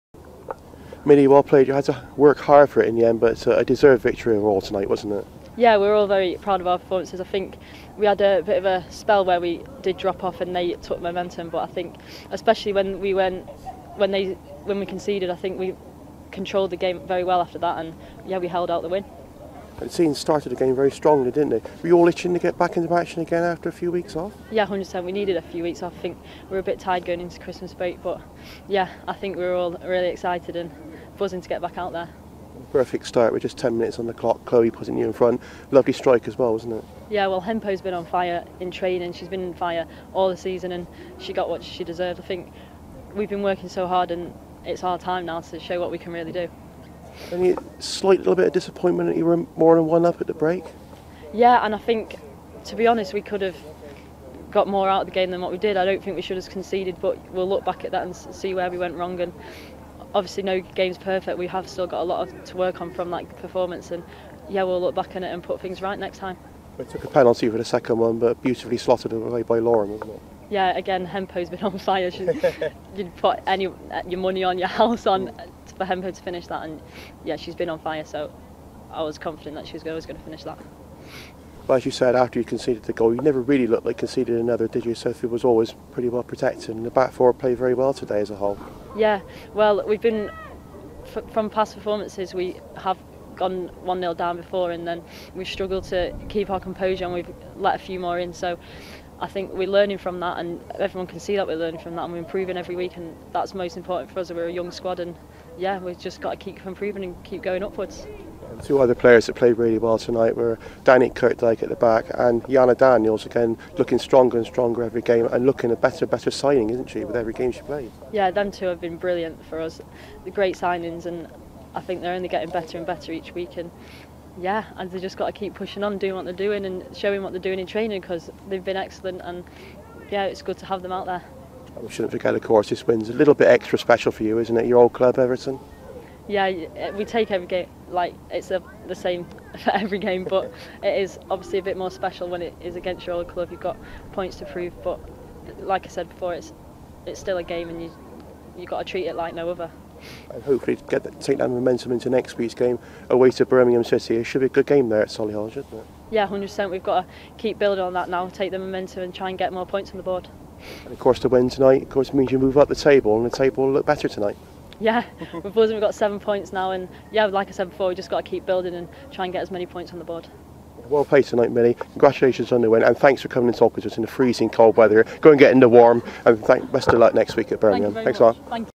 Millie Turner
Bristol City captain Millie Turner gives her reaction following their 2-1 WSL 1 victory over Everton.